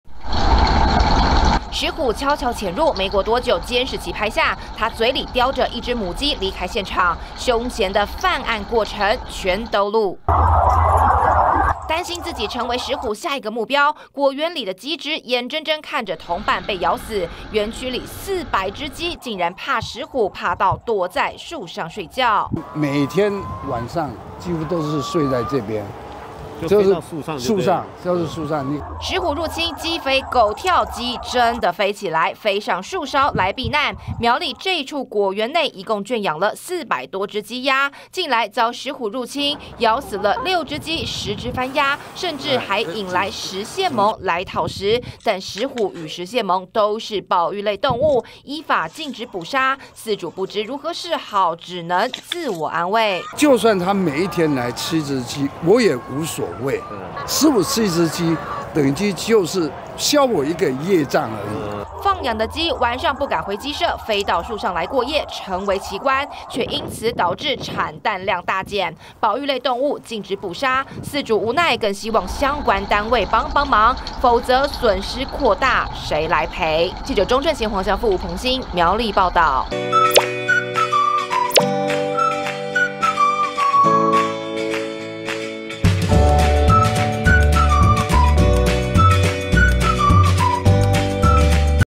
【每日必看】奇觀! 石虎咬死雞 400隻母雞嚇爆"躲樹上過夜" 20231202
石虎入侵果園，咬死放養的雞鴨！四百隻母雞眼睜睜看著同伴被咬死，嚇得晚上紛紛飛到樹上避難睡覺，也成為奇特景象，但也因為如此，母雞不下蛋，損失慘重！而石虎又是保育類動物，禁止捕殺，讓飼主頭痛不已。